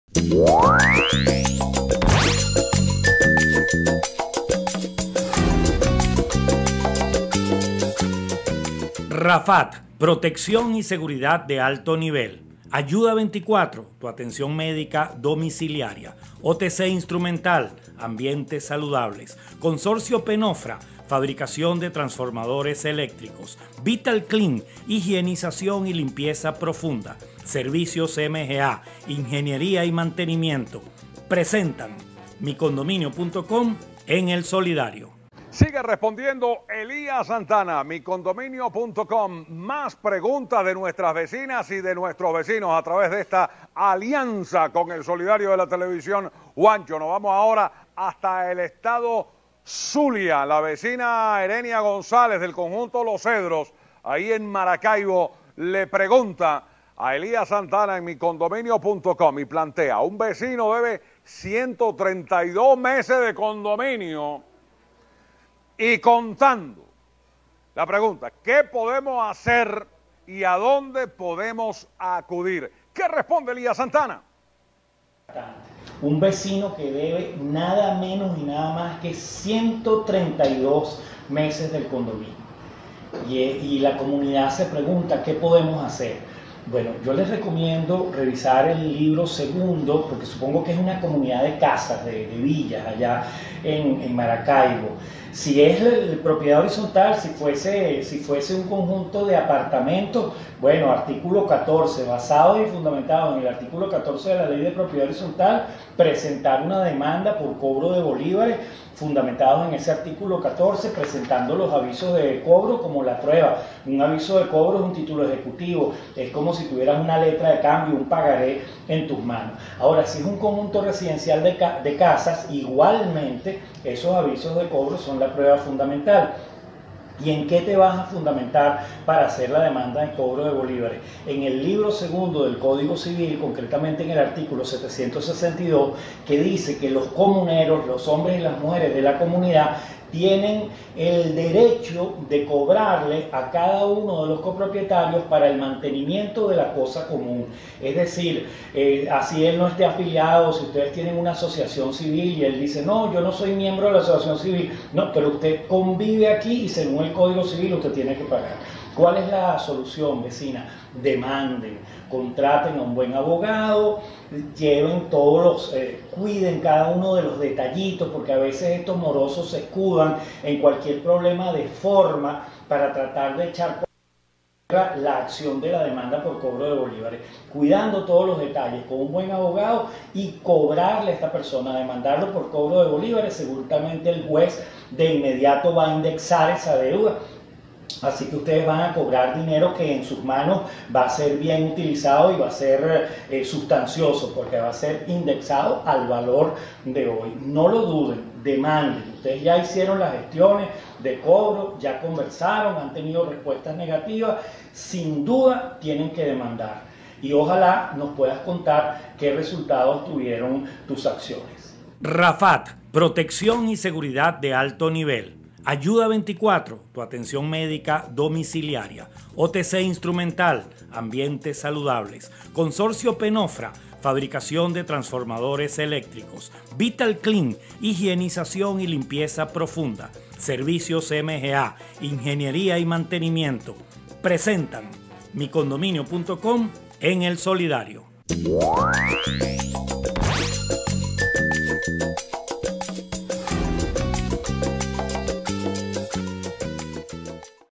Un vecino debe 132 mese de condominio y contando ¿Qué podemos hacer y a donde podemos acudir?
Caracas,Venezuela. Globovision. Miercoles 10 de Febrero 2021